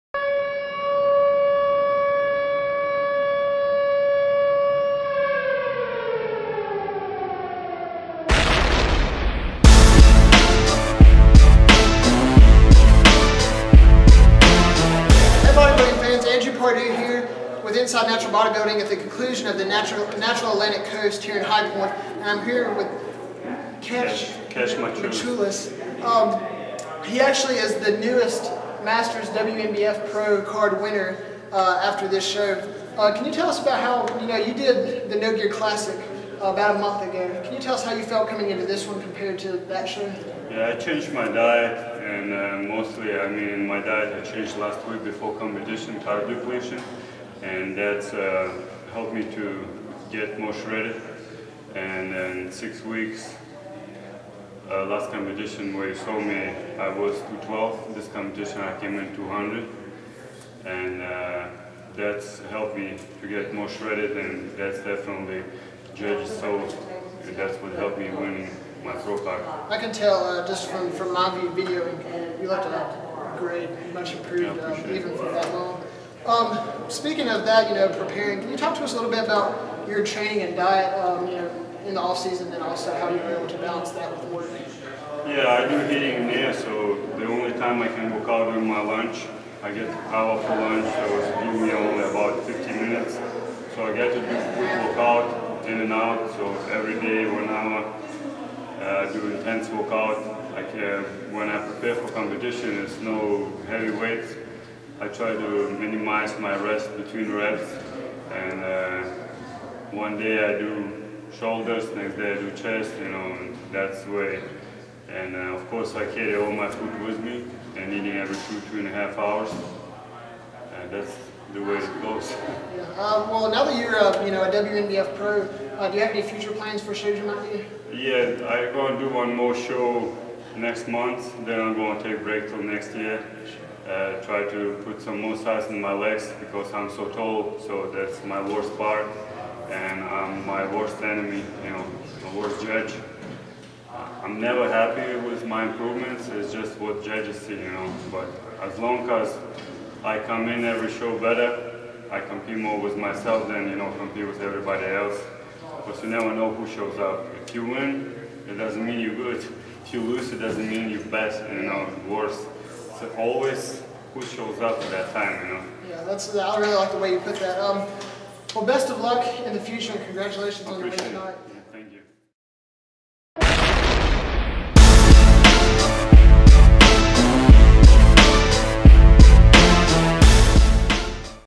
Newest WNBF Master's Pro Kestutis Maciulis at the 2013 Natural Atlantic Coast
2013 Natural Atlantic Coast held in High Point, NC